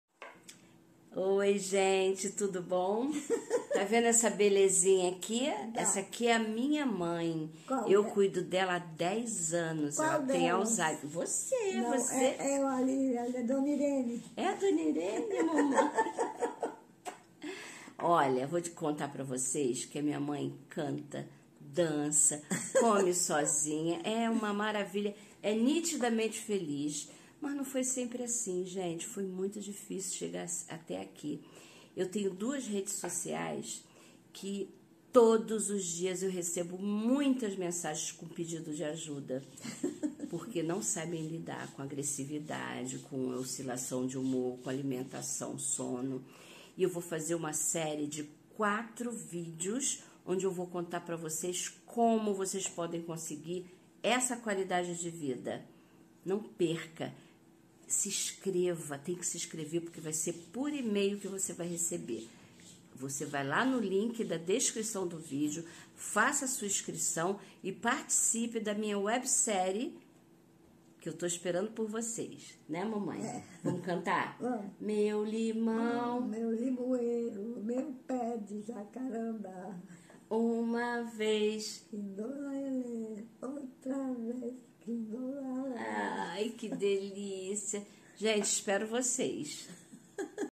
ALZHEIMER - Webserie leia a descrição do vídeo. 17/11/2020
Você já me conhece, a mamãe tem Alzheimer há  10 anos e está muito bem.
Come sozinha, se comunica, dança, se diverte e é feliz.
Mas não foi sempre assim.
Demorei pra entender porquê da alteração de humor, da agressividade, da falta de sono... E agora em que eu consegui  entender todas as nuances do Alzheimer, quero que você consiga também.
Fiz 4 vídeos pra te ajudar. Eles são on line e gratuitos.
Se inscreve aqui